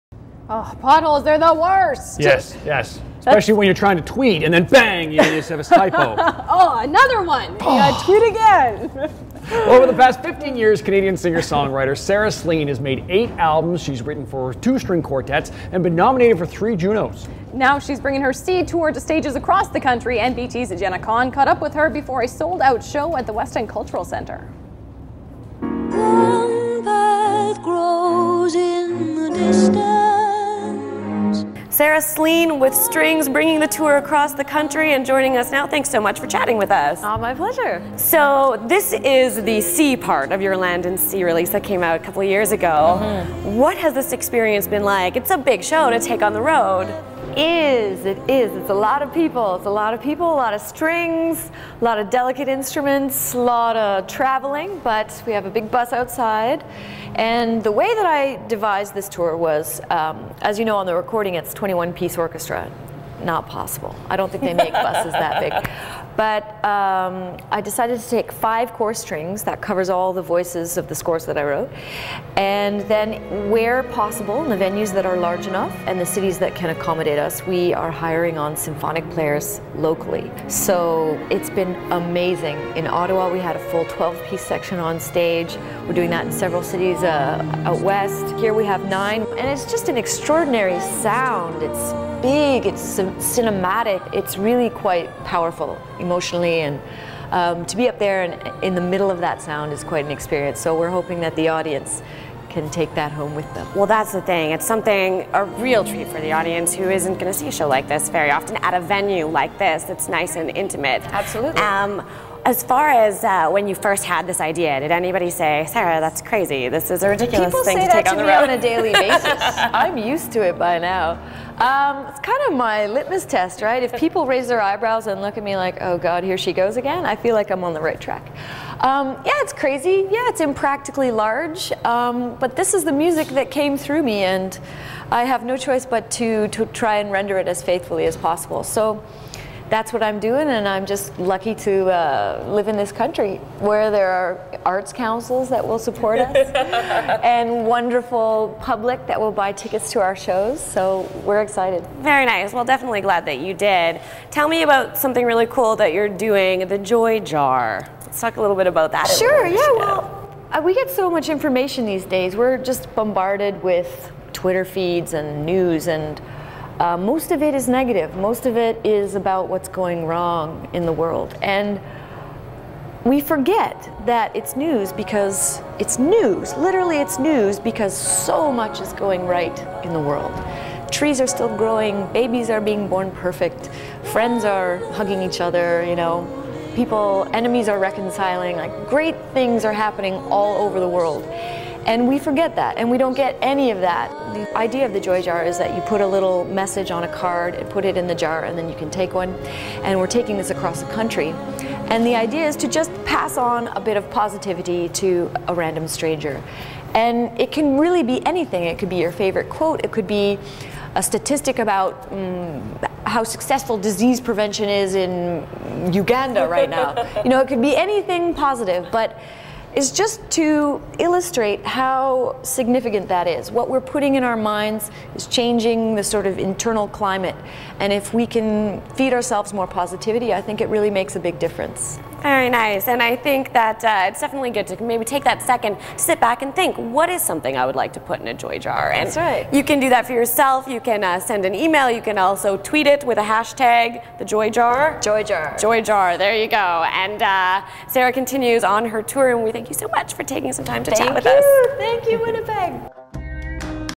Sarah Slean
Jenna Khan catches up with Canadian singer songwriter Sarah Slean while she was here in Winnipeg